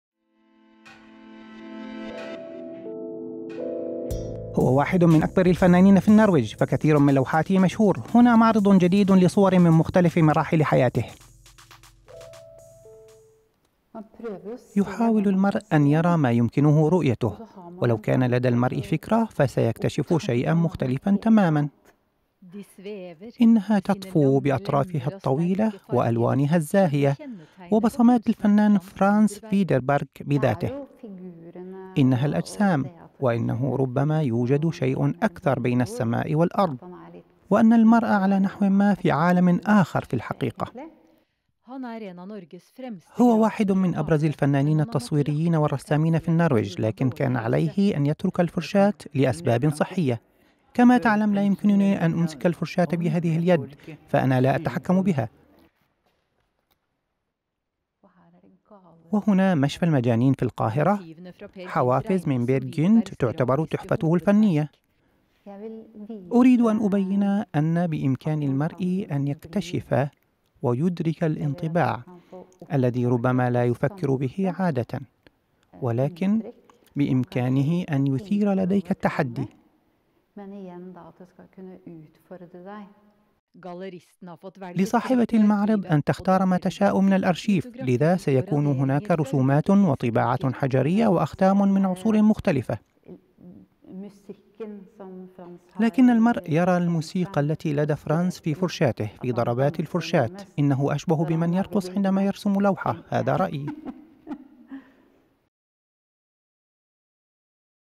Frans Widerberg, norsk grafiker الفن التصويري
هو واحد من أكثر الفنانين في النرويج . فكثير من لوحاته مشهورة .
هنا معرض جديد لصور من مختلف مراحل حياته!